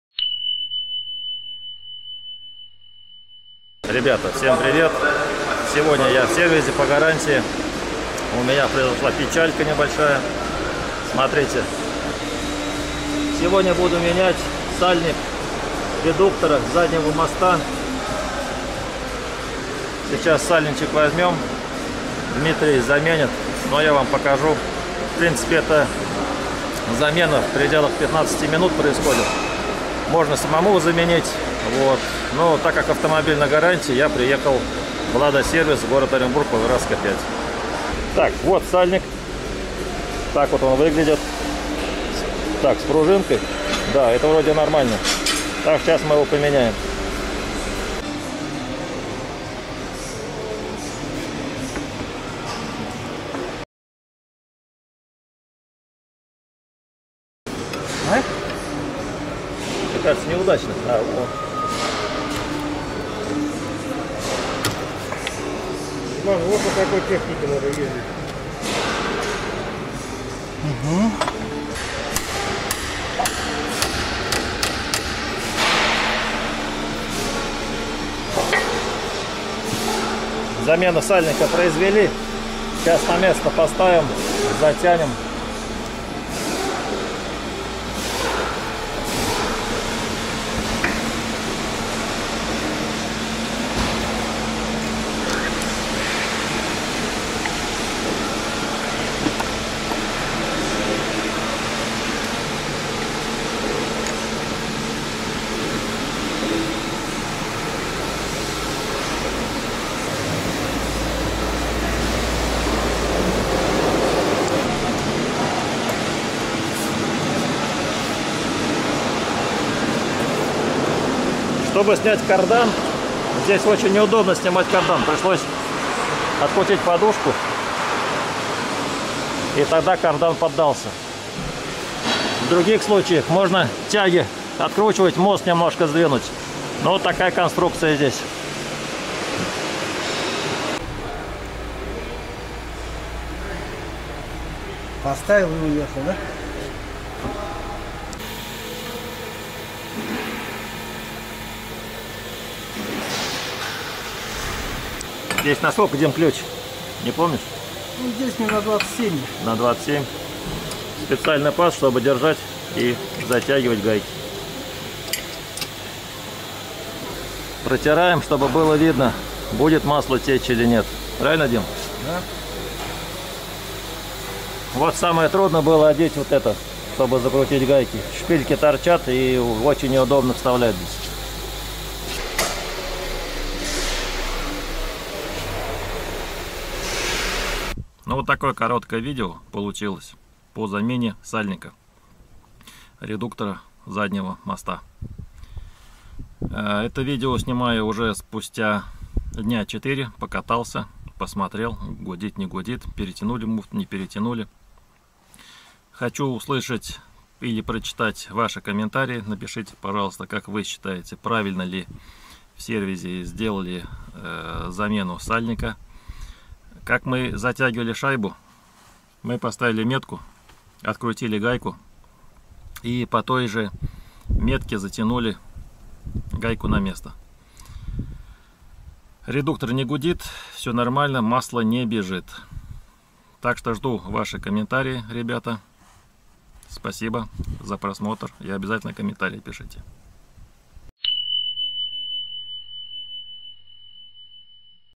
🔥 ЗАМЕНА САЛЬНИКА РЕДУКТОРА ЗАДНЕГО МОСТА НИВЫ / REPLACING THE REAR AXLE GEARBOX OIL SEAL 🔥
1.  ЗАМЕНА САЛЬНИКА РЕДУКТОРА ЗАДНЕГО МОСТА НИВЫ.

2.  НА РАЗВИТИЕ КАНАЛА : СБЕРБАНК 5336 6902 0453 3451 ВИТАЛИЙ СЕРГЕЕВИЧ О

5.  Яндекс Эфир